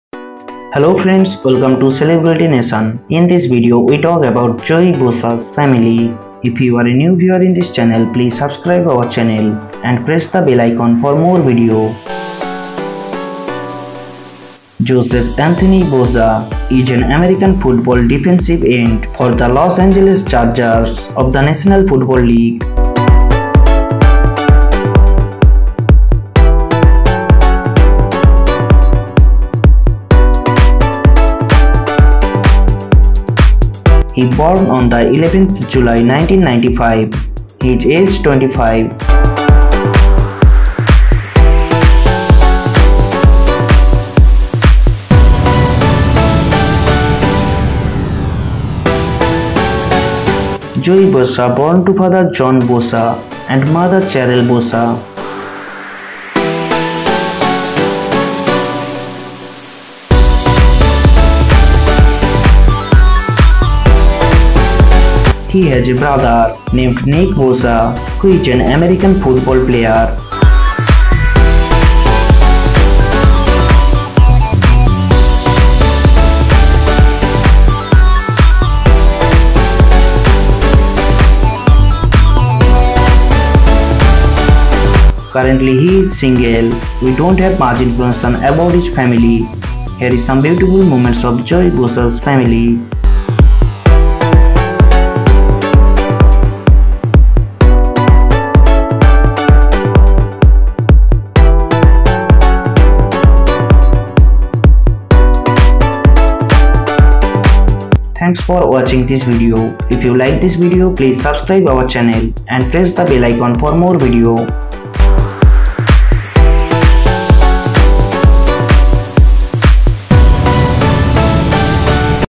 Joey Bosa Family With Father,Mother and Brother Nick Bosa 2020
Real Name : Joseph Anthony Bosa
Born : 11 July 1995 (Age 25)
Occupation : American Football Defensive End
Team : Los Angeles Chargers

Here is Joey Bosa's Family Member List :

Father : John Bosa
Mother : Cheryl Bosa
Brother : Nick Bosa